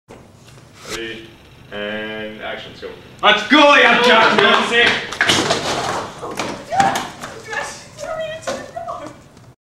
into the door
law project. leah and i are about to get beaten up by scary ira guys with sheleighlies (sp?). but leah ruins the PERFECT take by whining "you just threw me into the door!"